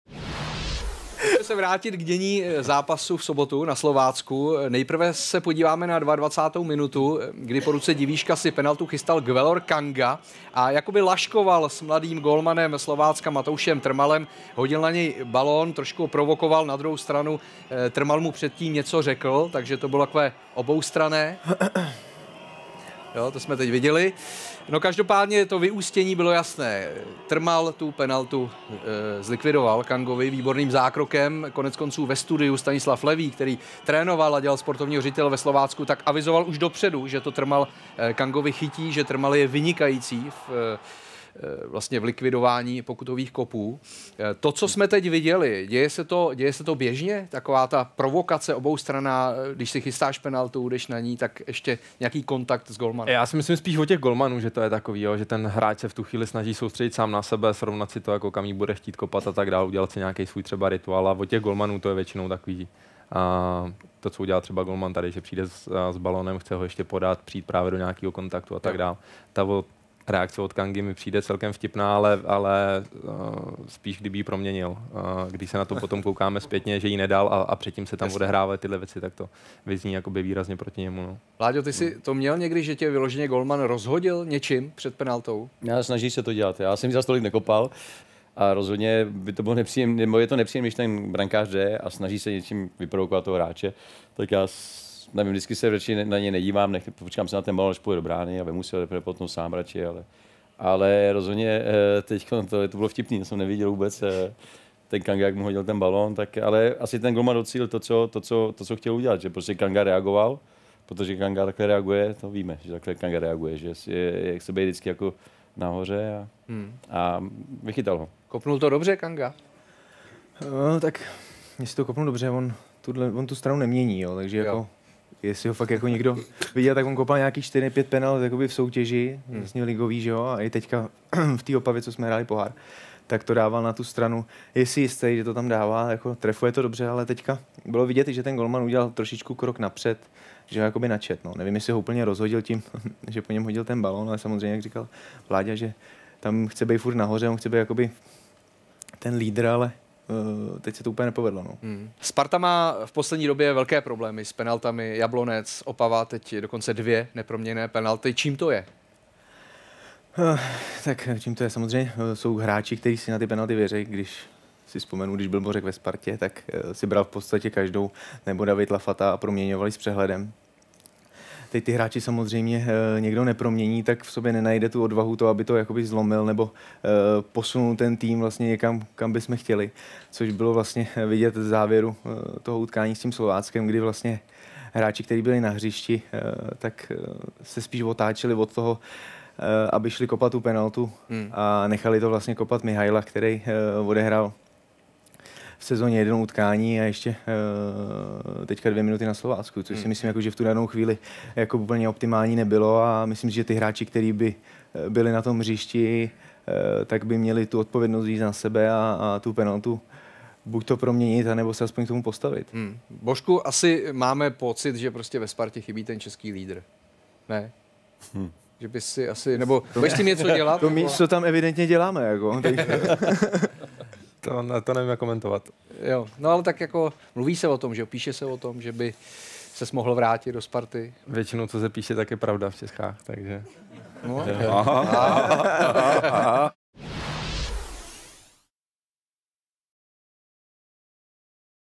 TIKI-TAKA: Bořek Dočkal o návratu do Sparty
Jak hosté naší TIKI-TAKY viděli menší rozmíšku, ke které došlo mezi záložníkem Sparty Guélorem Kangou a brankářem Slovácka Matoušem Trmalem? A jakými slovy komentoval Bořek Dočkal svůj možný návrat na Letnou?